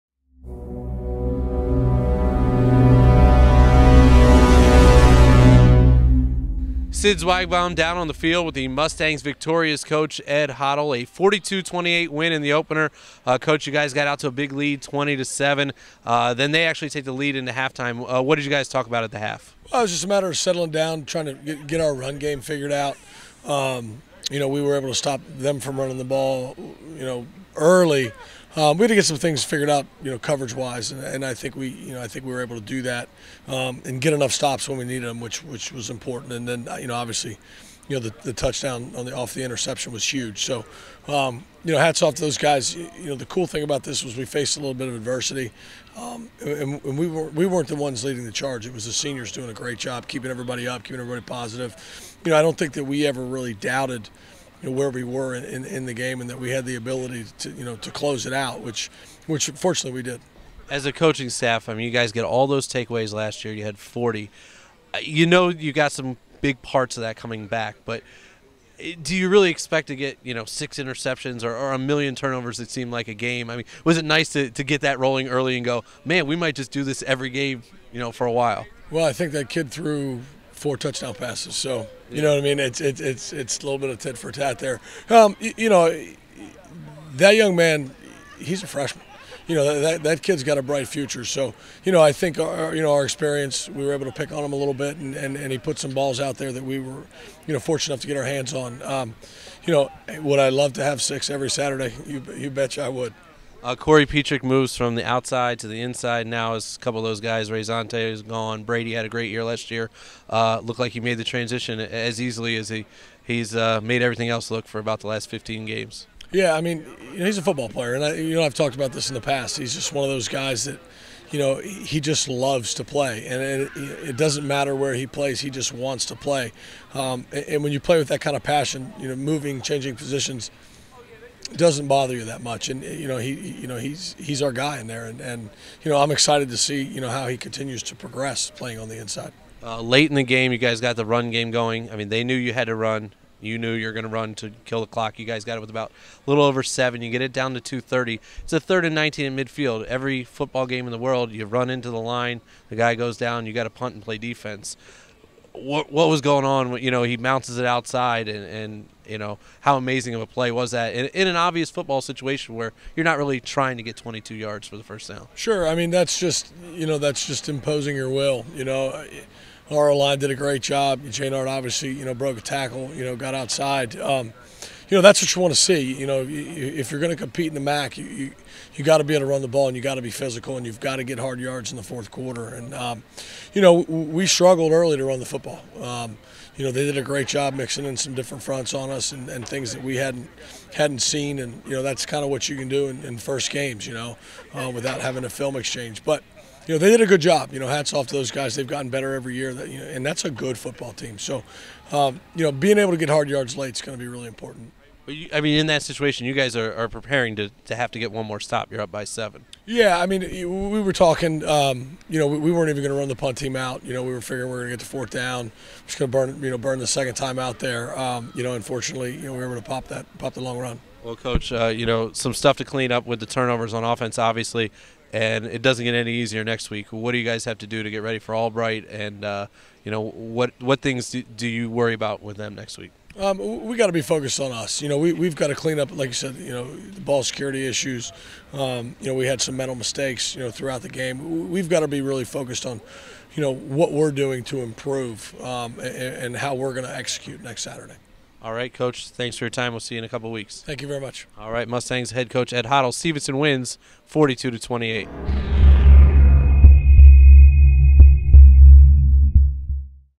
Football vs NC Wesleyan Ed Hottle Postgame Interview
Stevenson's Head Coach talks about the team's season opening 42-28 victory over NC Wesleyan Friday night at Mustang Stadium.